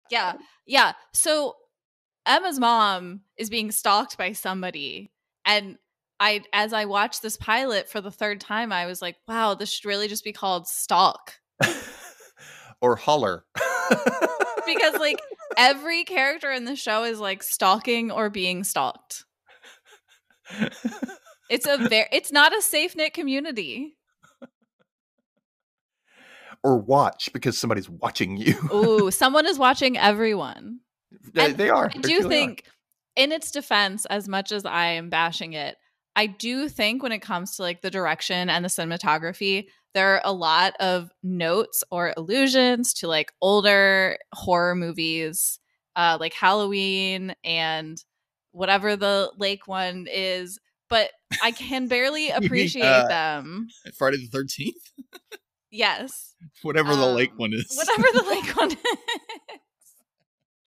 The Rewatch Recap podcast clip - MTV Scream S1E1: Pilot (“Red Roses”)
Aurelien asks: Should this really carry the infamous Scream title or should it be named something else entirely? 😅

Available Now Where Most Podcasts Are Played!